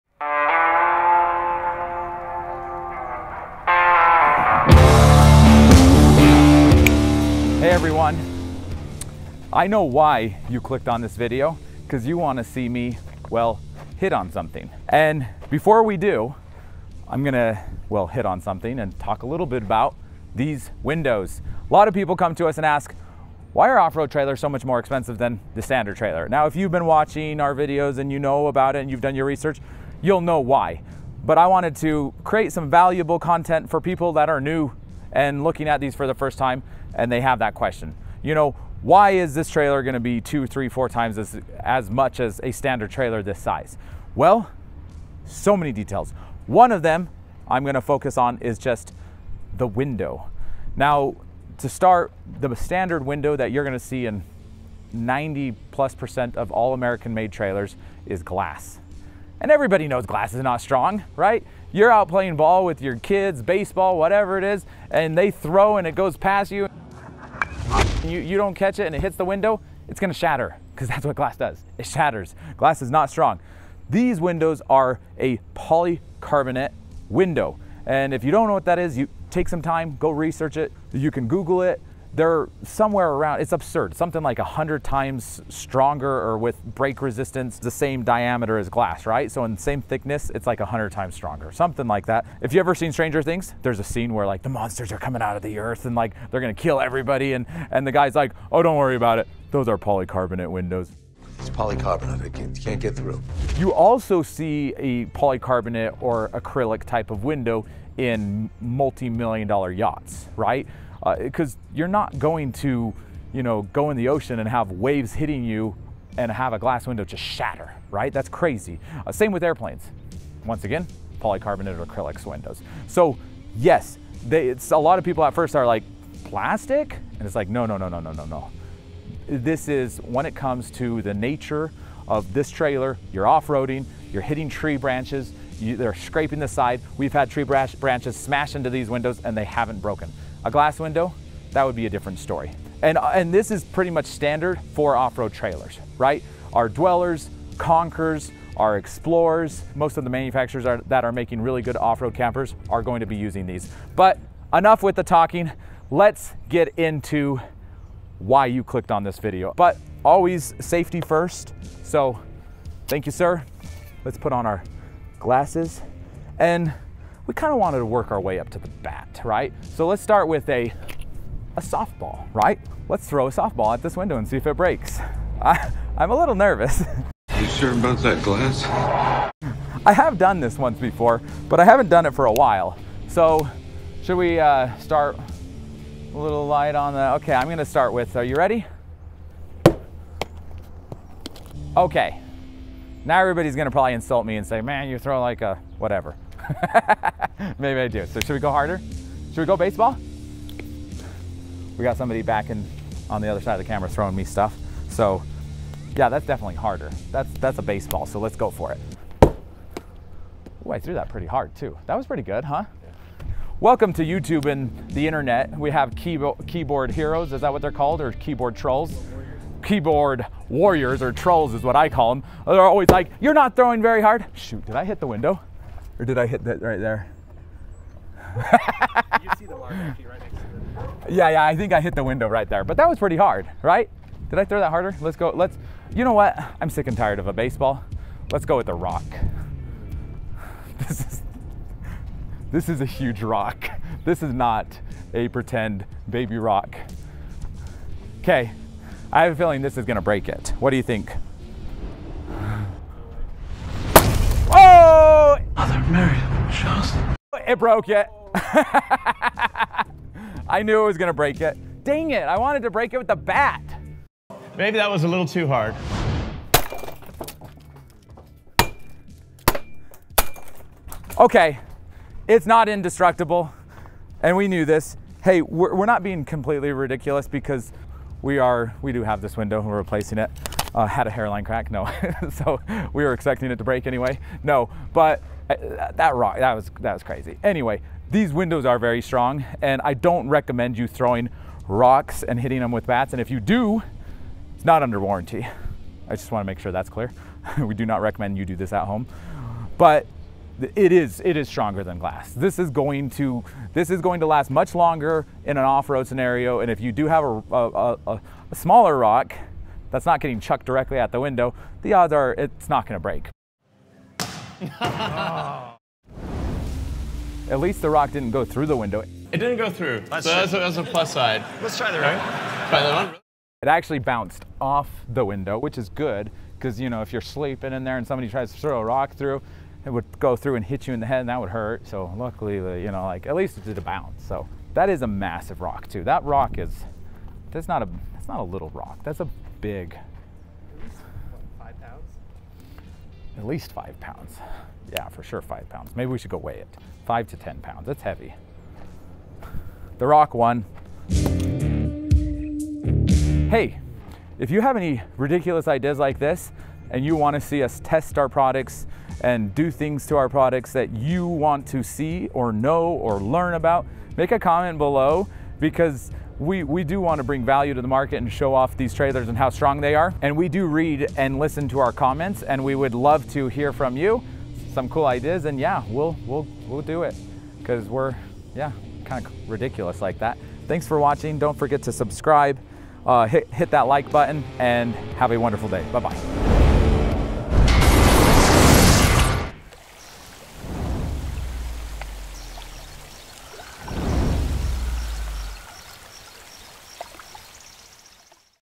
CRAZY Quality Test On Our OFF-ROAD Trailers | EUROVISION OBI Dweller Window Test | ROA OFF-ROAD
If you have ever wondered how strong polycarbonate window are... this is for you! In this video test it to the MAX.  FIND out how they hold up to stress testing all the way up to it's breaking point!
watch to see how much these off-road trailer polycarbonate windows can take!

We're always trying to come up with new fun ideas for future videos that bring you the most value.

For more info on our camper trailers, visit our

Use of this footage outside of ROA Off-Road/RVs Of America is prohibited without written consent.